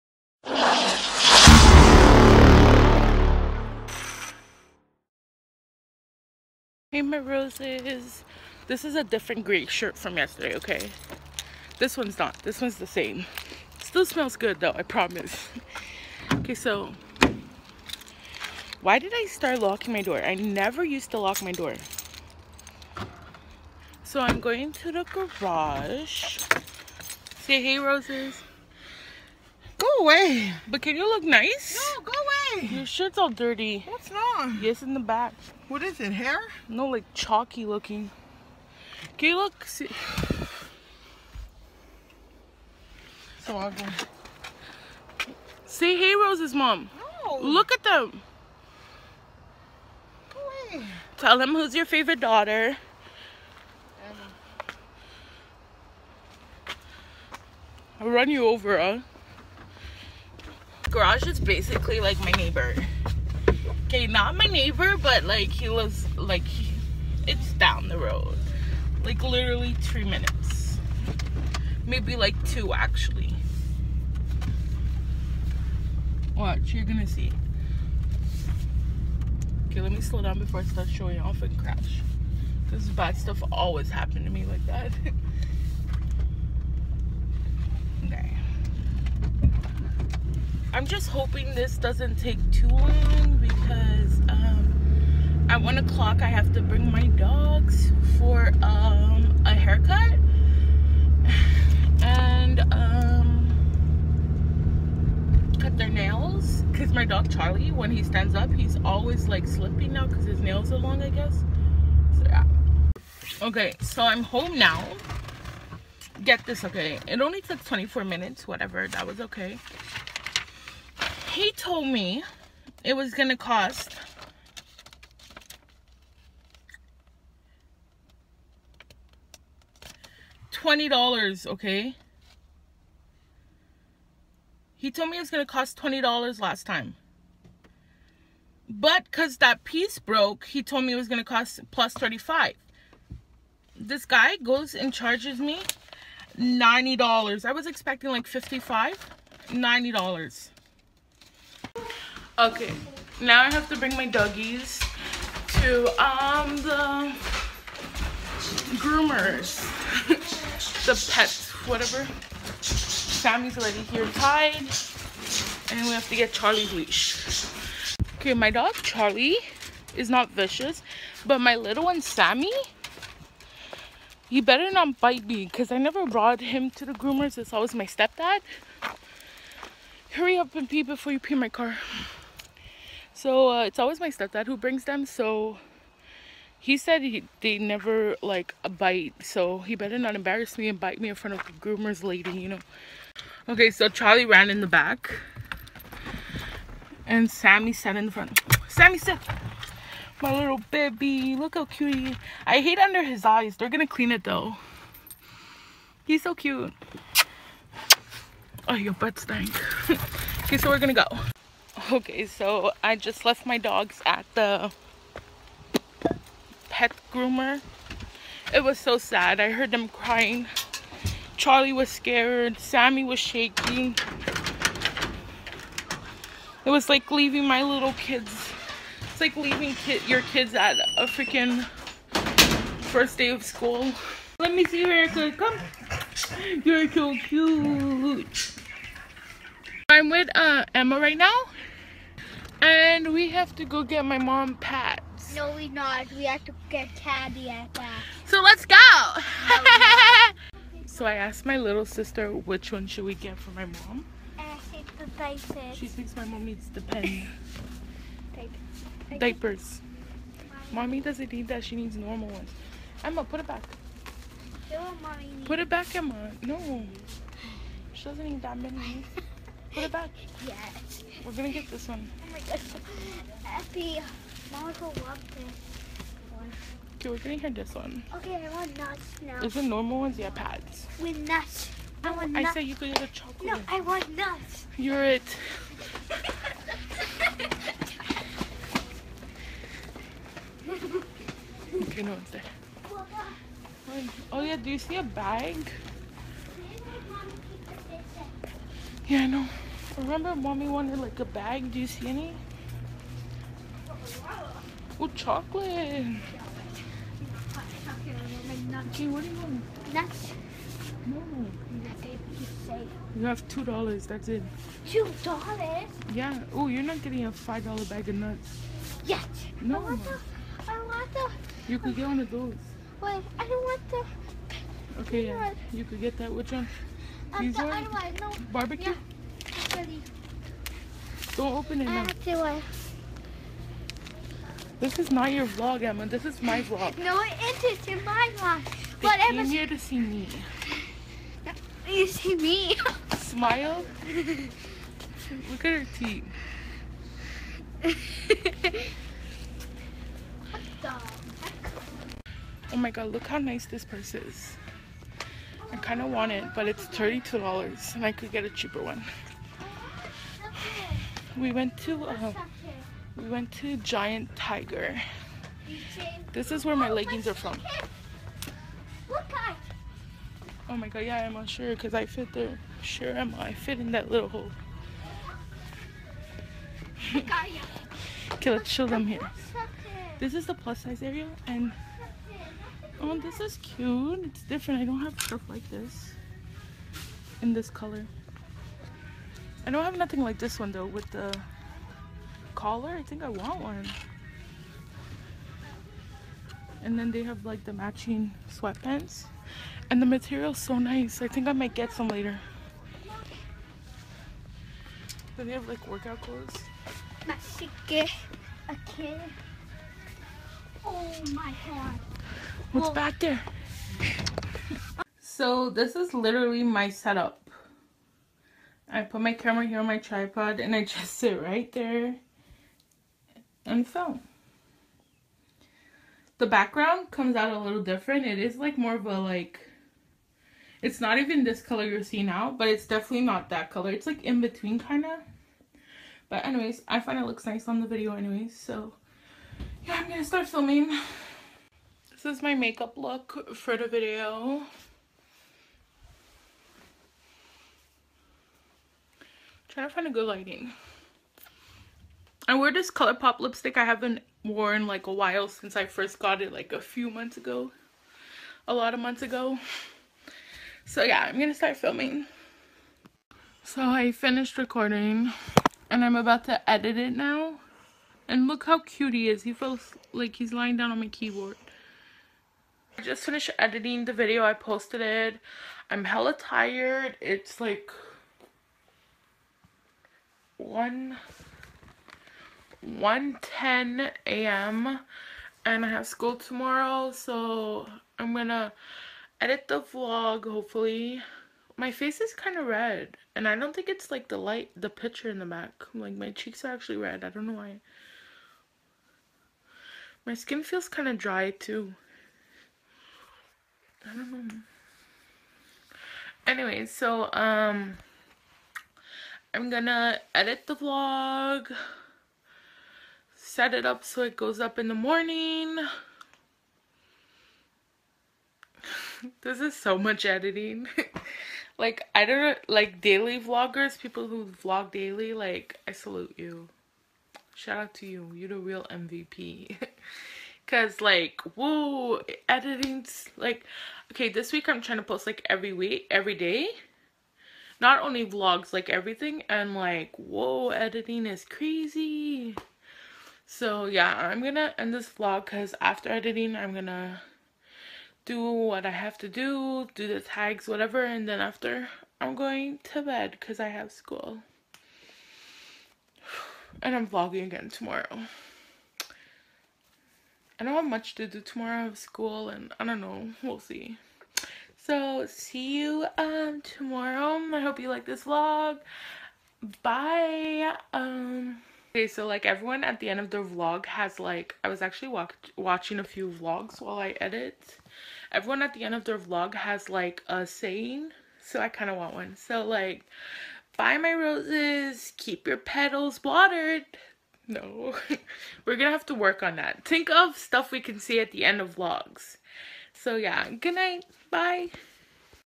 GOING TO THE GARAGE & SHOPPING [SEASON1 EP.25] |RosaBonadonnaTV
HEY ROSES, SO IN THIS VLOG I WENT TO THE GARAGE GET MY BACK LIGHT FIXED WITH I THINK WAS HELLA PRICEY BUT WTV THEN I JUST GO OUT ALITTLE.